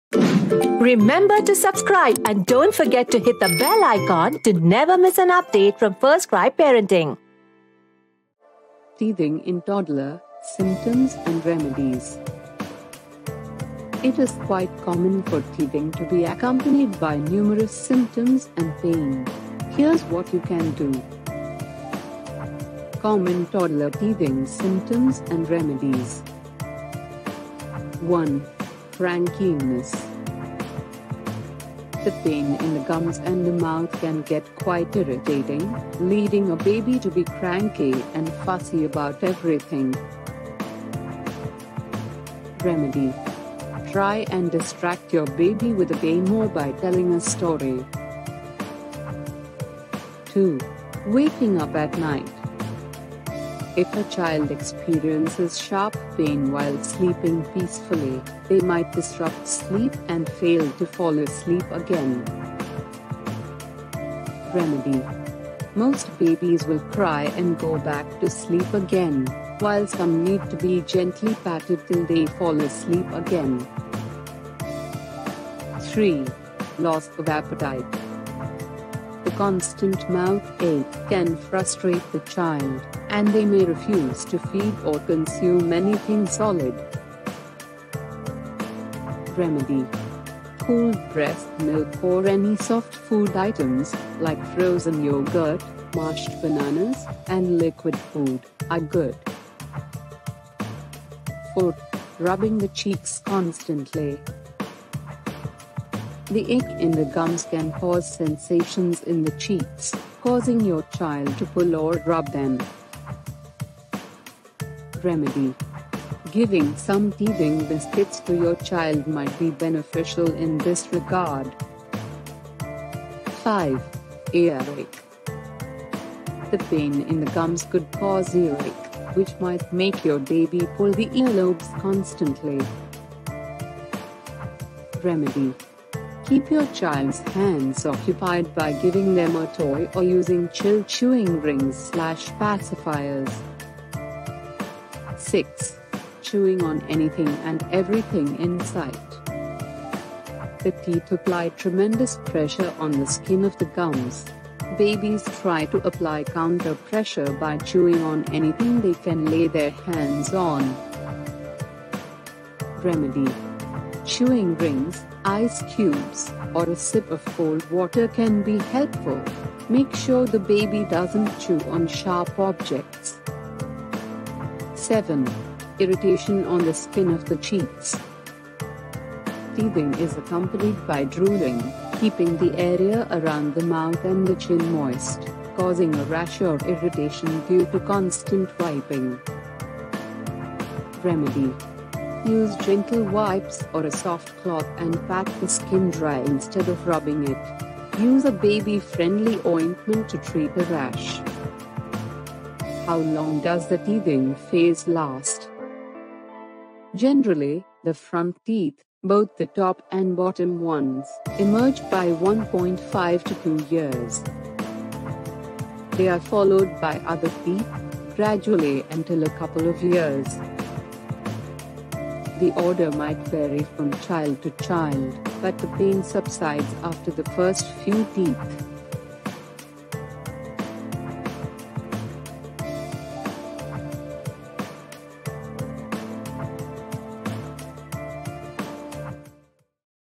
Teething in Toddler – Symptoms & Remedies
Teething is a vital developmental milestone in a child’s life, but it can be quite frustrating for the little one and also for the parents! During the teething in toddlers, they may have disturbed sleep, wake up frequently at night, drool, and have a mild temperature. You can gain more understanding about teething signs in toddlers with the help of this video.

Most babies start teething when they are about six months old, but some may begin teething when they turn one. In toddlers, it is usually the molars that are awaited. When little ones get new teeth, it’s pretty painful for them as the teeth push their way to emerge out of the soft, tender skin of the gums. But these are normal signs of teething, and the discomfort does fade away after some time.